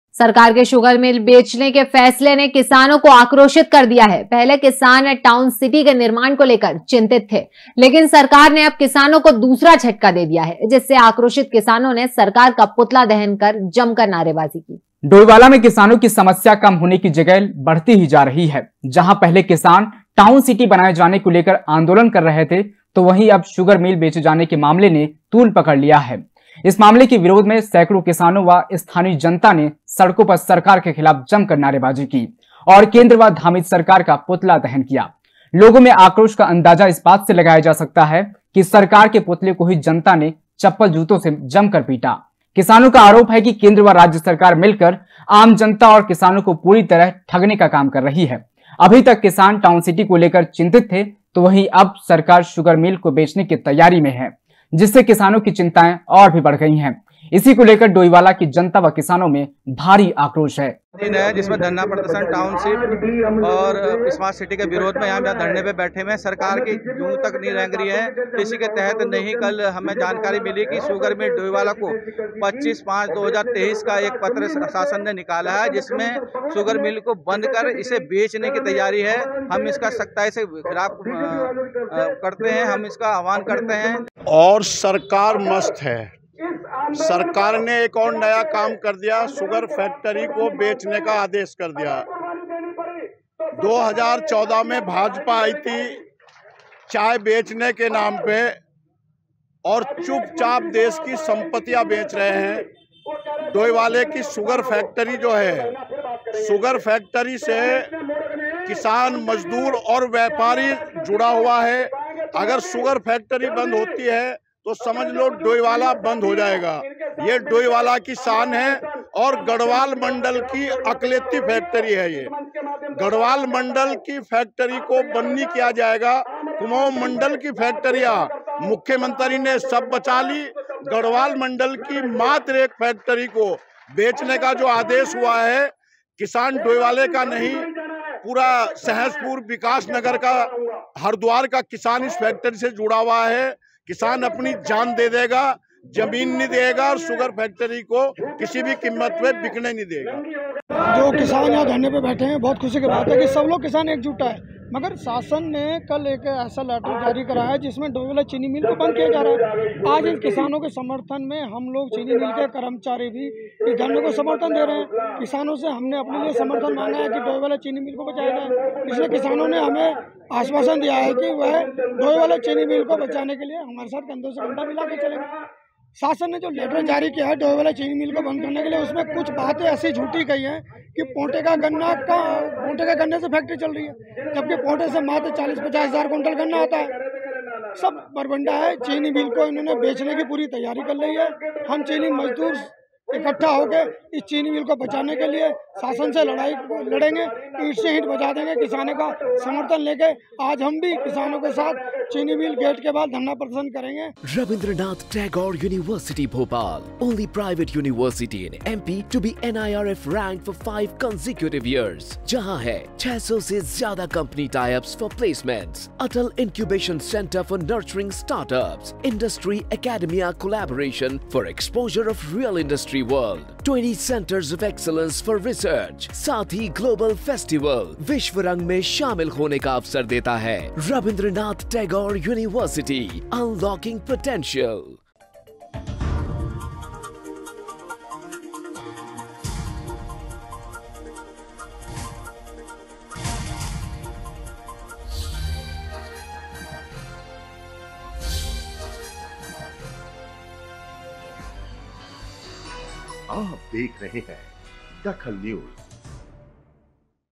DAKHAL NEWS शुगर मिल बेचने से किसान हुए आक्रोशित
शुगर मिल बेचने से किसान हुए आक्रोशित
सरकार के खिलाफ जमकर की नारेबाजी